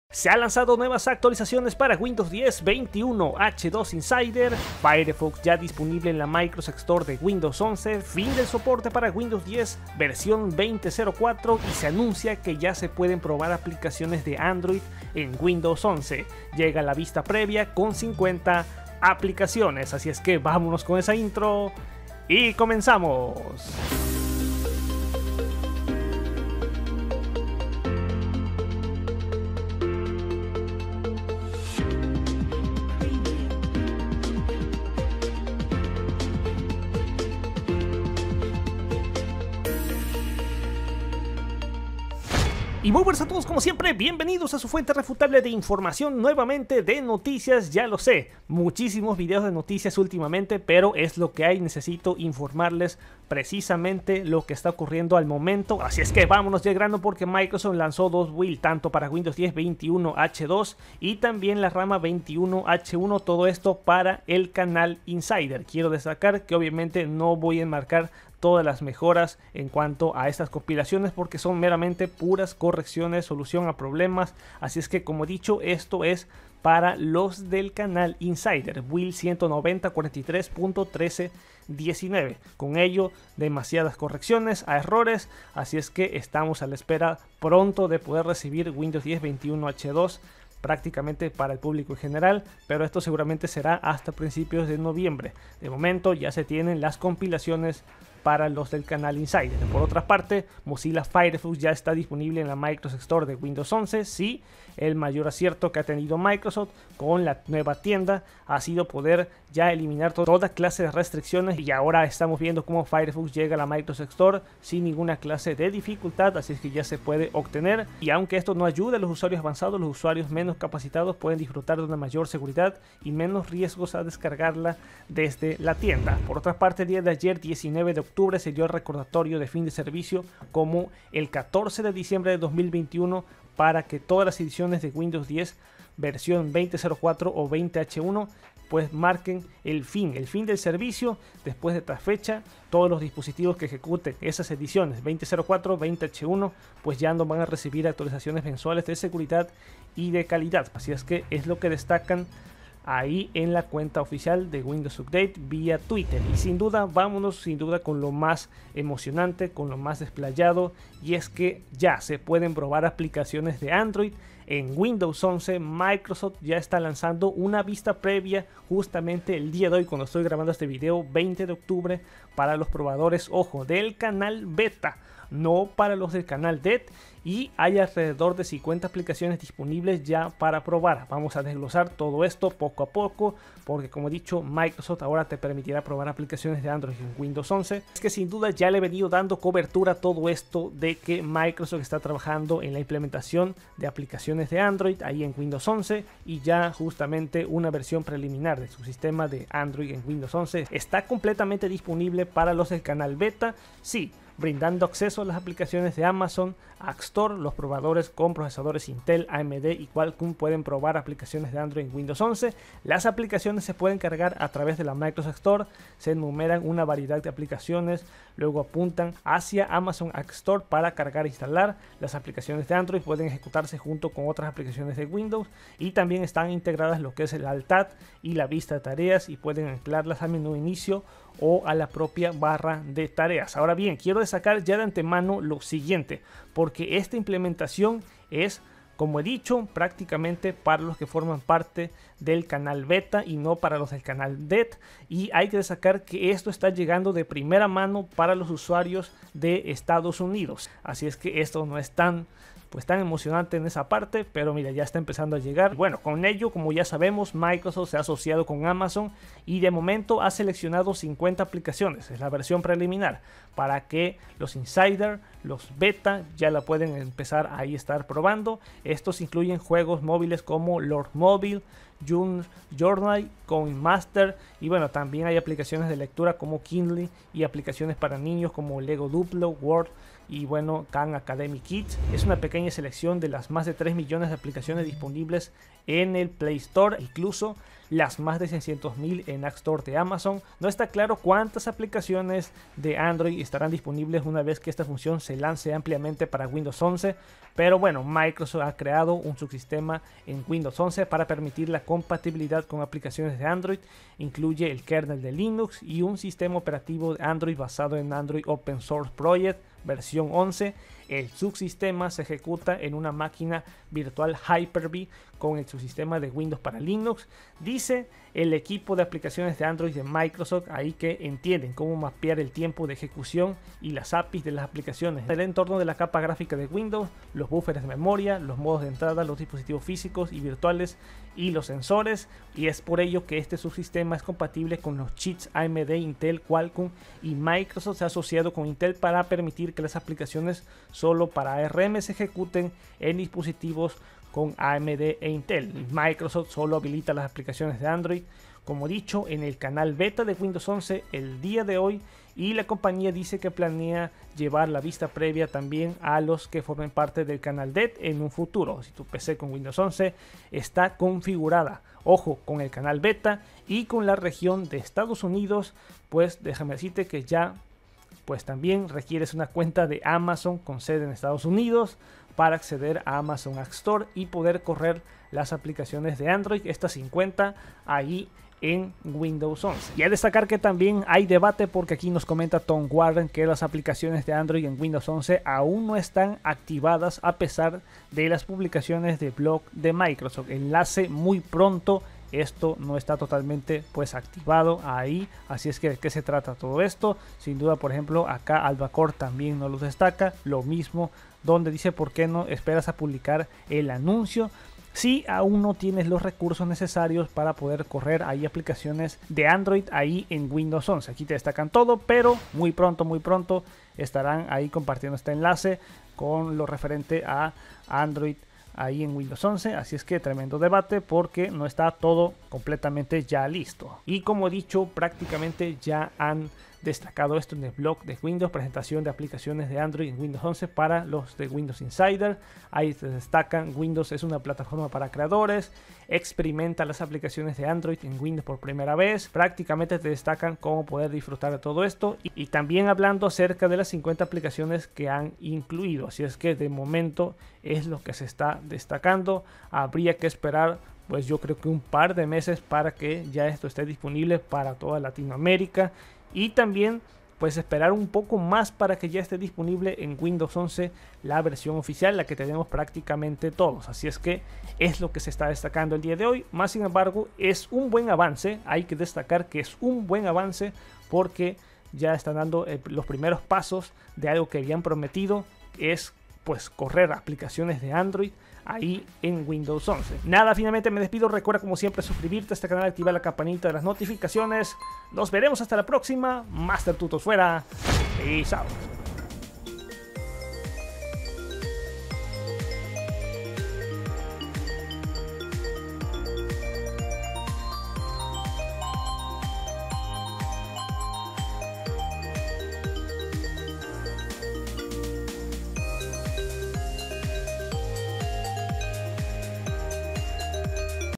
LANZAN⚡APPS de Android en Windows 11 / Ya LISTO! SUBSISTEMA de Windows para ANDROID
⭐Ya esta empezando la implementación de Apps de Android para Windows 11 y con ello el subsistema de Windows para Android comienza su implementación para el Canal BETA

🔥Requisitos a contemplar para correr las nuevas Apps de Android en Windows 11 🔥

💠Tu PC debe tener Windows 11 (compilación de la serie 22000.xxx) y cumplir con estos requisitos de hardware.
💠Es posible que deba habilitar la virtualización para el BIOS / UEFI de su PC.
💠Asegurate de estar ejecutando Microsoft Store versión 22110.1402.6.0 o superior.
💠La región de su PC debe estar configurada en EE. UU.
💠Su PC debe estar en el Canal Beta. Si es nuevo en el programa Windows Insider
💠También necesitará tener una cuenta de Amazon con sede en EE. UU. Para usar Amazon Appstore.